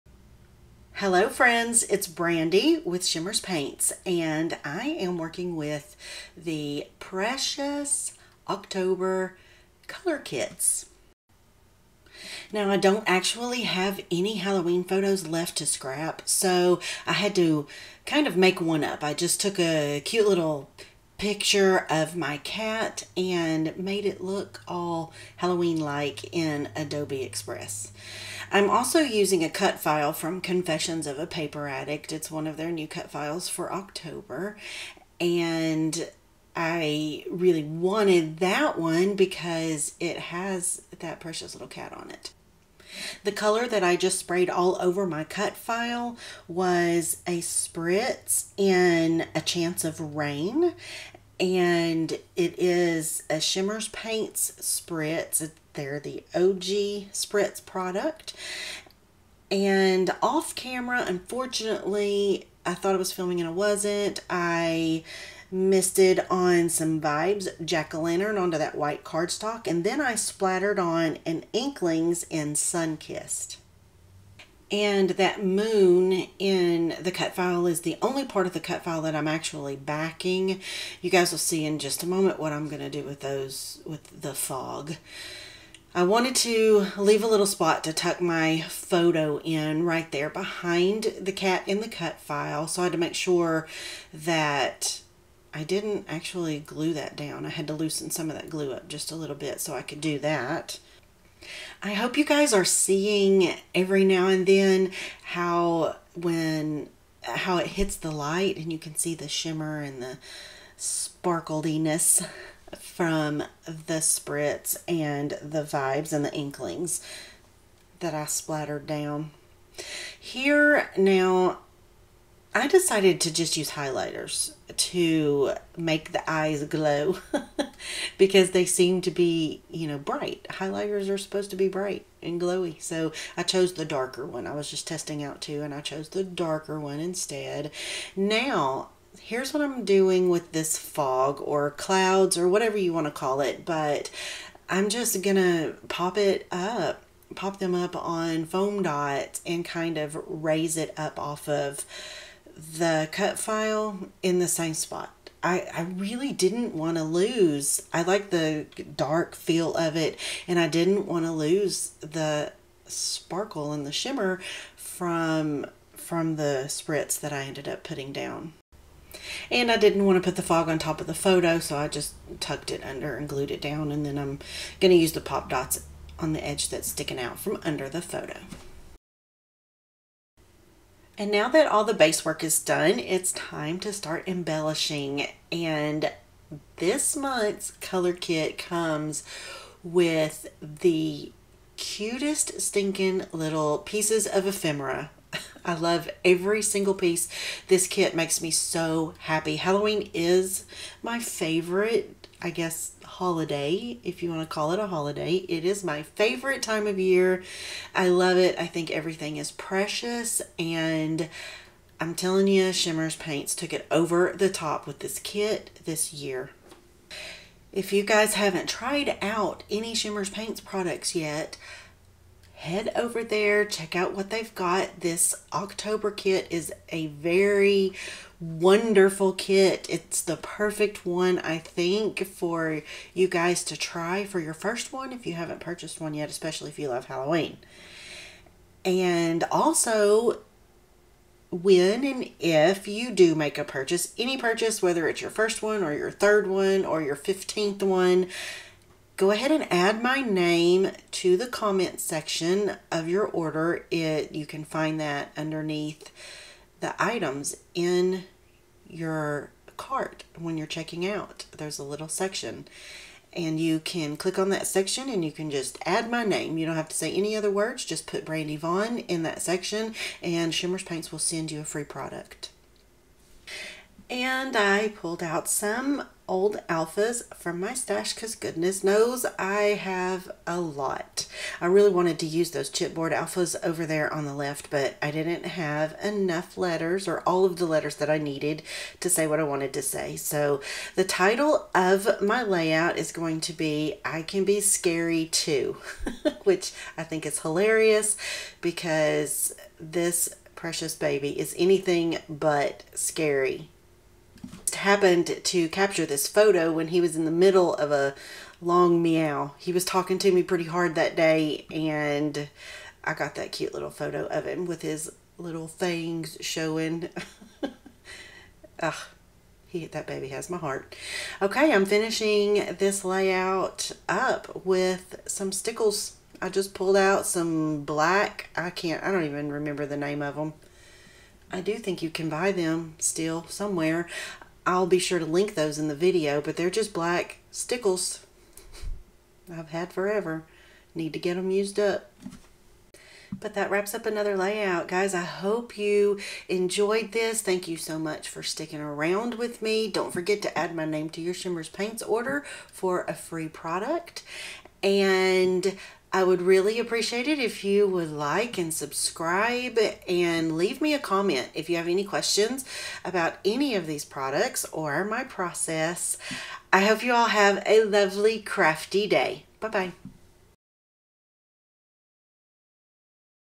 I CAN BE SCARY TOO | SHIMMERZ PAINTS OCTOBER COLOR KITZ (Process Video # 79)
Thank you for joining me for my newest process video using the October Color Kitz from Shimmerz Paints.

Check below for links to the Shimmerz FB and IG accounts as well as the online store.  Don't forget to add my name, Brandy Vaughn to your order for a FREE paint product!

xoxo,
Brandy

xoxo
Brandy